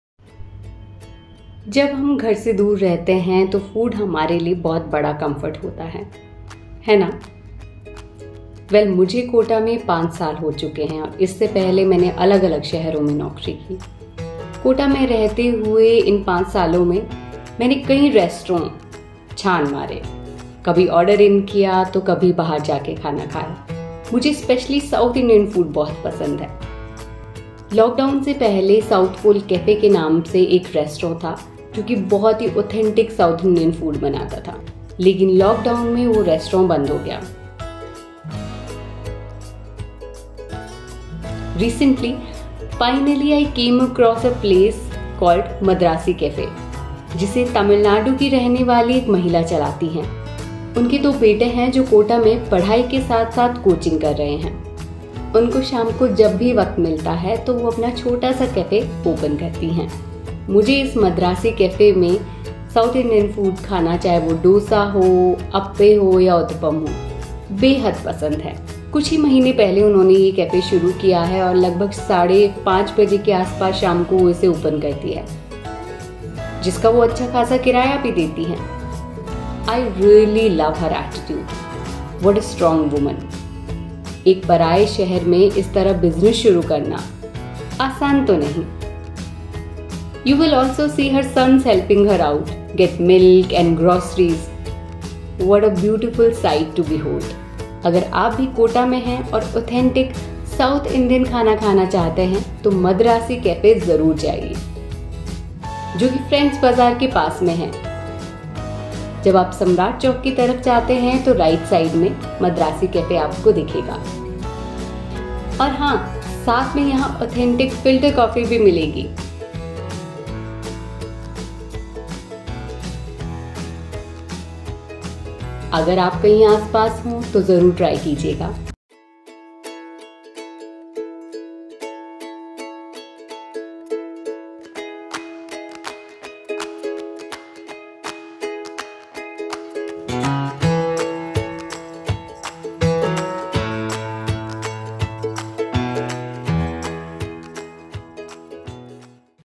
Madrasi Cafe | Madrasi Cafe in Kota | Kota Rajasthan
Recently came across the Madrasi Cafe in Kota, here you can have authentic south Indian Food. This is run by a woman from Tamil Nadu. If you are in Kota do check out this place.